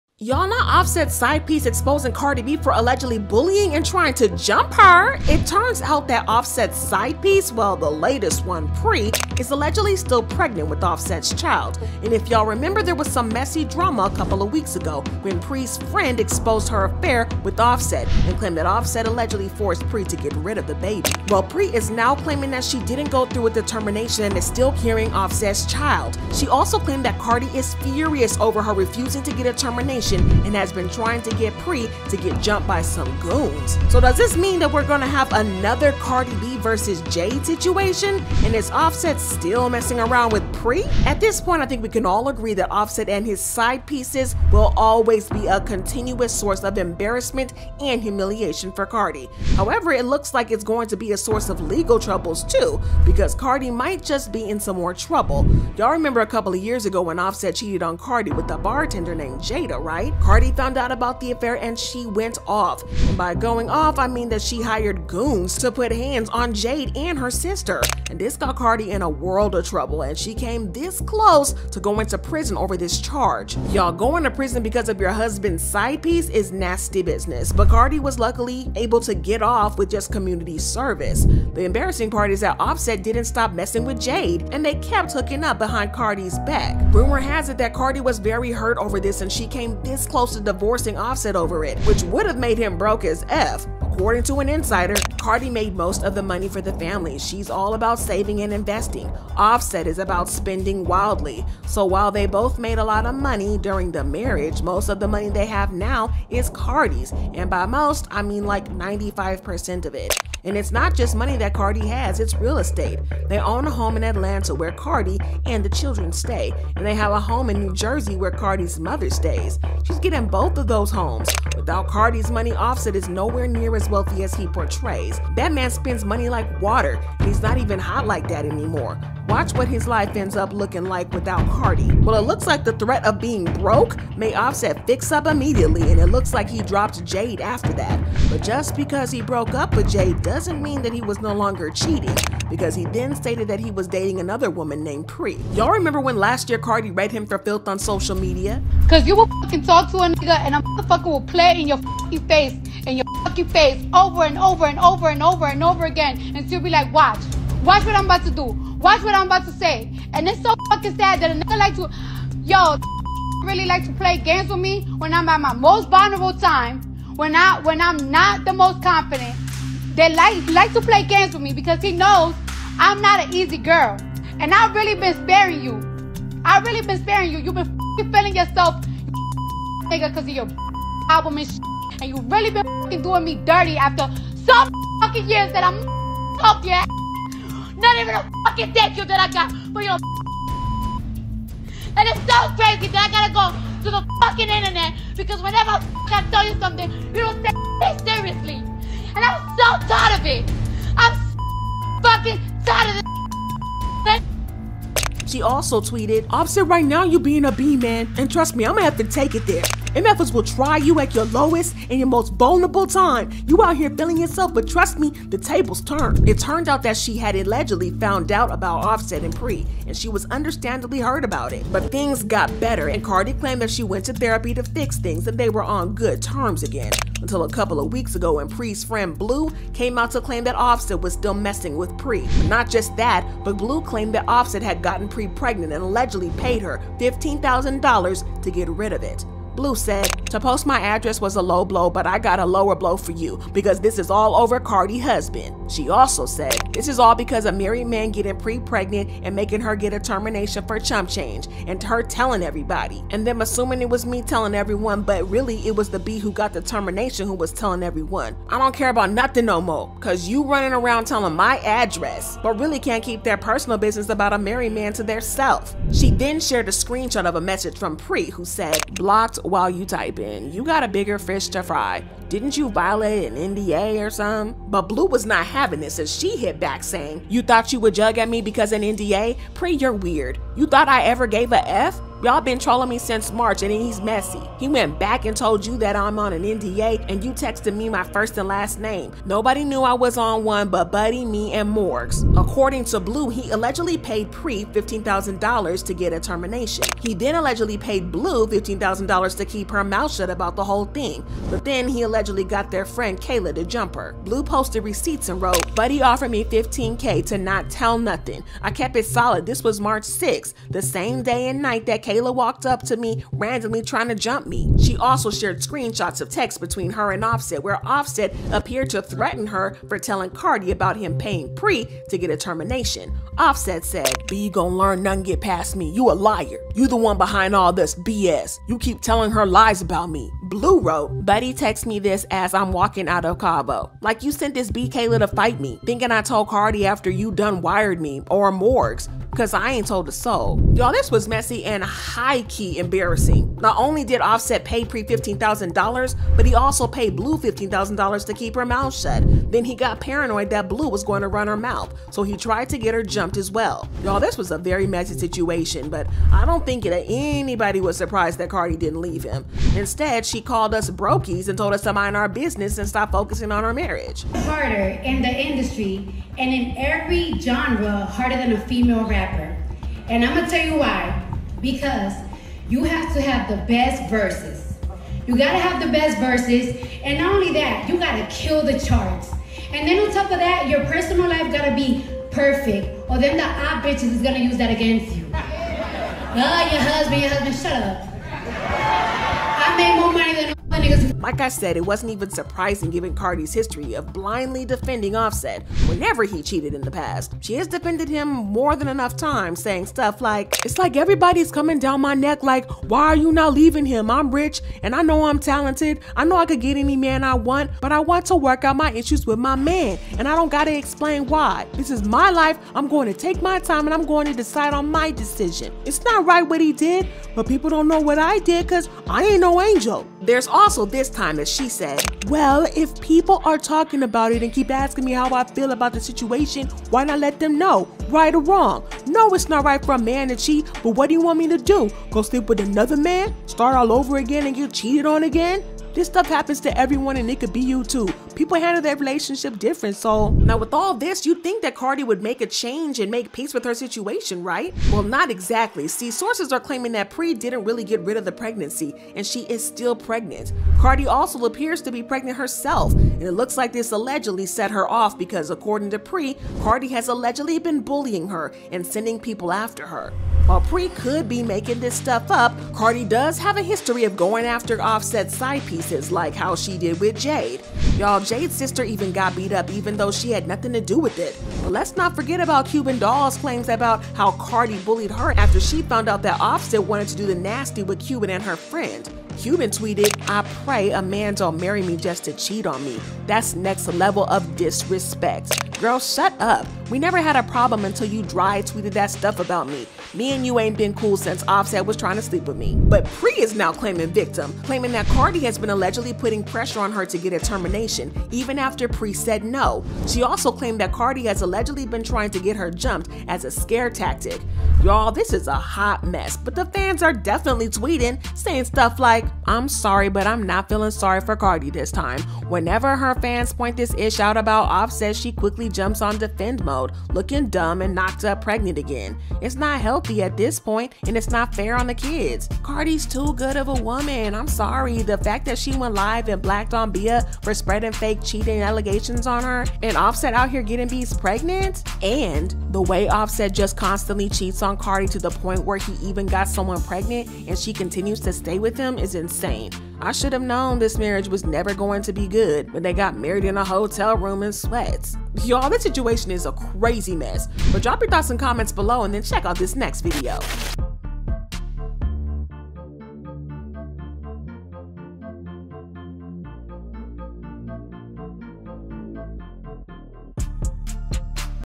Offset's Side Piece SLAMS Cardi B For Bu!lying Her | Cardi Set Her Up To Be Jumped?
Offset's Side Piece SLAMS Cardi B For Bullying Her | Cardi Set Her Up To Be Jumped?

Y’all, not Offset’s side piece exp0sing Cardi B for allegedly bu!lying and trying to jump her! It turns out that Offset’s side piece… well, the latest one, Pree, is allegedly still pregnant with Offset’s child. If y’all remember, there was some messy drama a couple of weeks ago when Pree’s friend exp0sed her affair with Offset and claimed that Offset allegedly forced Pree to get rid of it.

Well, Pree is now claiming that she didn’t go through with the termination, and is still carrying Offset’s baby. She also claimed that Cardi is FURIOUS over her refusing to get a termination and has been trying to get Pree to get jumped by some goons.

So does this mean that we are about to have another Cardi B vs Jade situation? And is Offset STILL messing around with Pree?